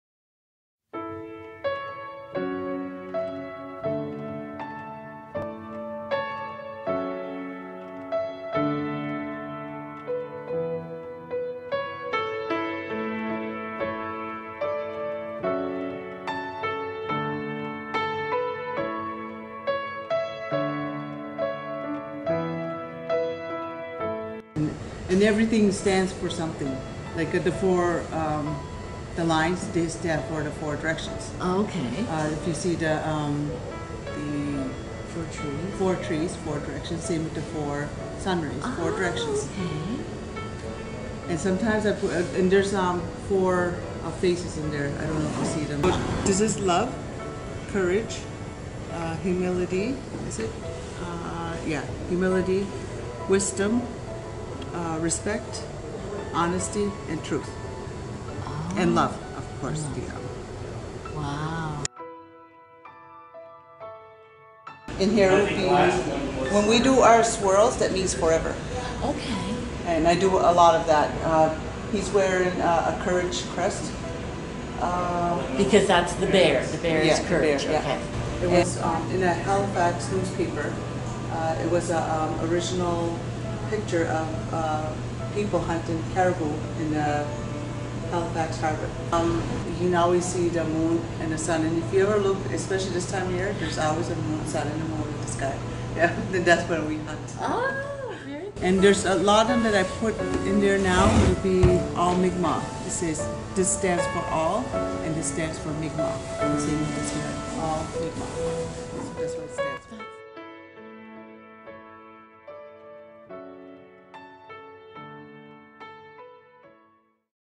Loretta Gould, Mi'kmaq Artist
Loretta Gould is a painter from Wagmatcook First Nation.